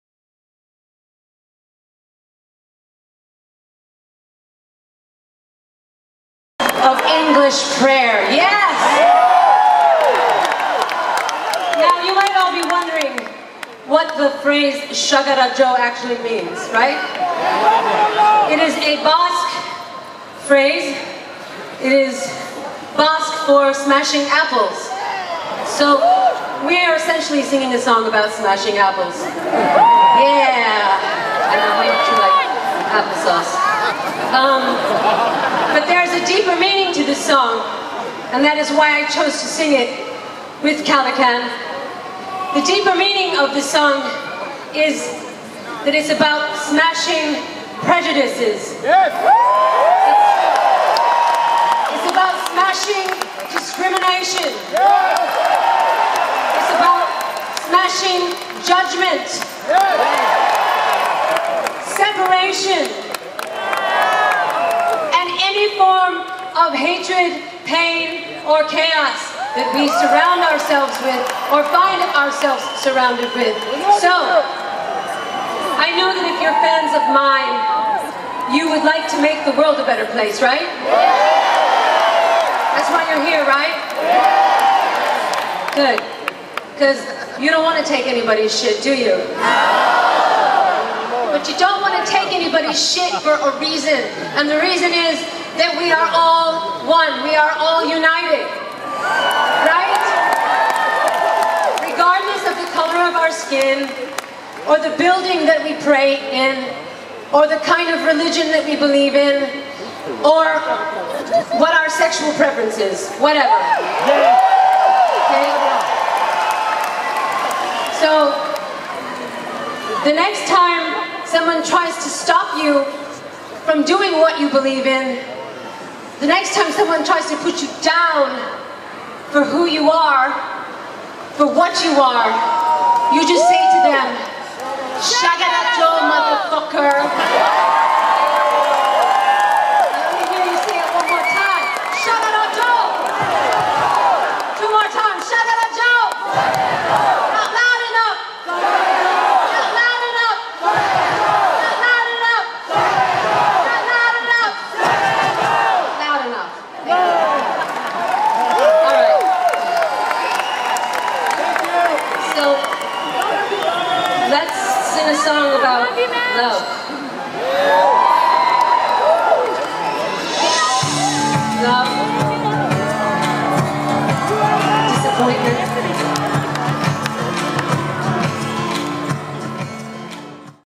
Hyde Park Speech
Madonna's speech during her July 17 MDNA Tour concert in Hyde Park